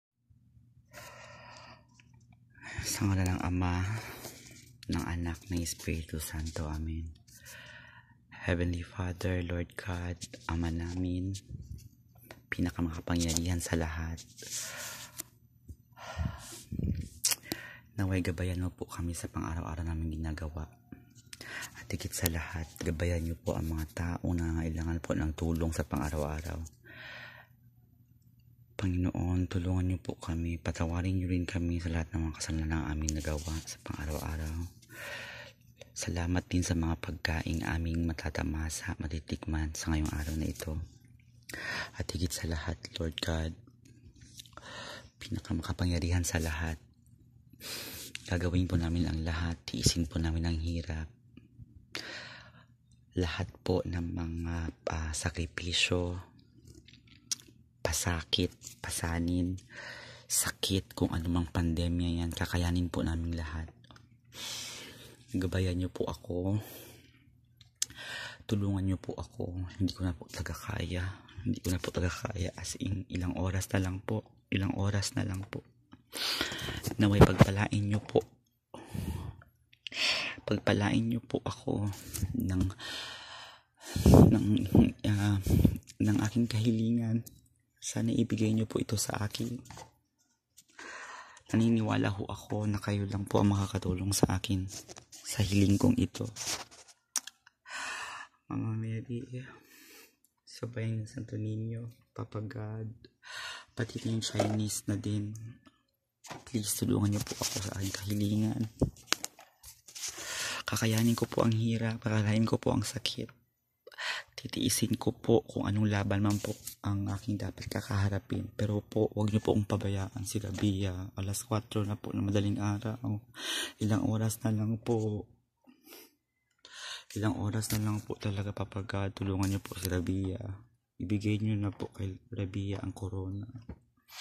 MISS UNIVERSE 2020 LAST MINUTE PRAYER PRAYER BEFORE START
I'M WILLING TO GIVE UP ONE DAY OF MY LIFE NOT TO BE - GUIDED BY GOD BUT I PRAY WITH ALL MY HUMB) E HEART YOU CAN IGNORE ME FOR TODAY LORD GOD HOWEVER PLEASE DONT LEAVE RABIYA'S SIDE UNLESS THE CROWN IS ON HER HEAD ALREADY!